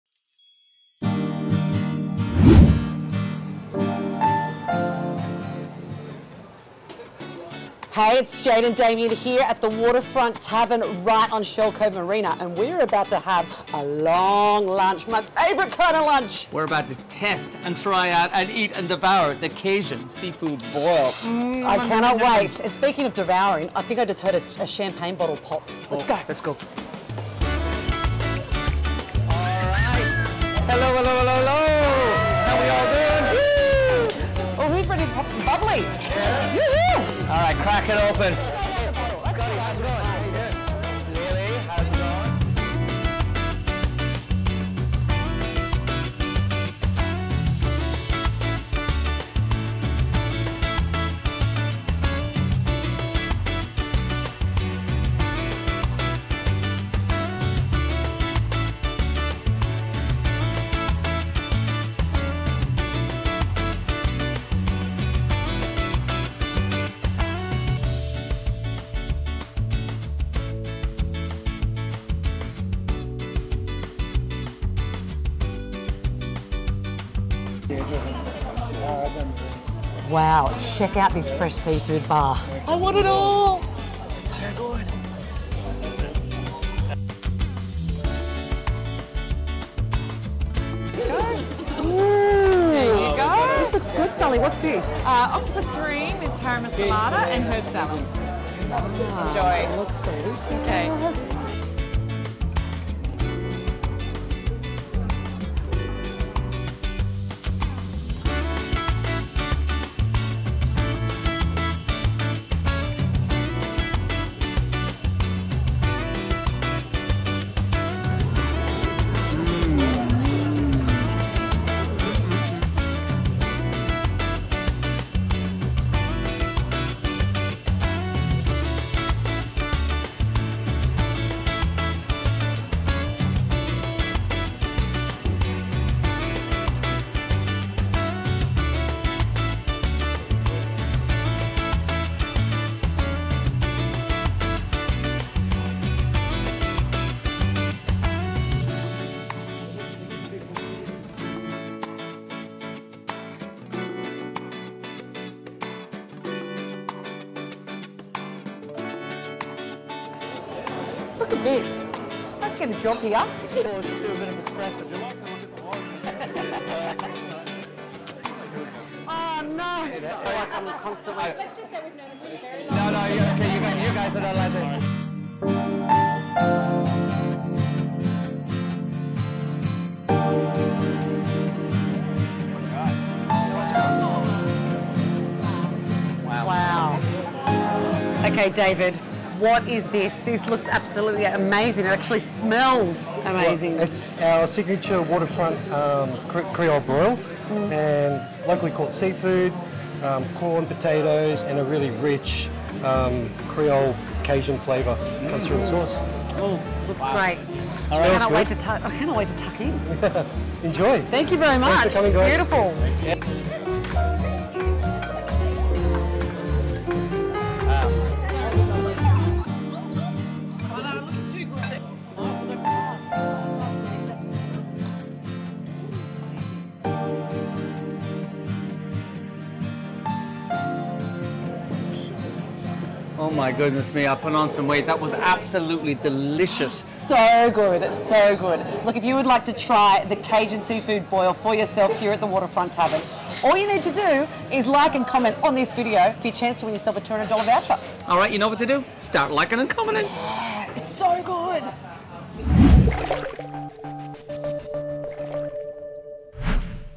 Jade & Damien's Long Lunch
Win your lunch like Jade & Damien put on for their long lunch!
Jade & Damien took 10 guests to the Waterfront Tavern to try their signature seafood boil. Watch as it is poured into the middle of the table for them to share.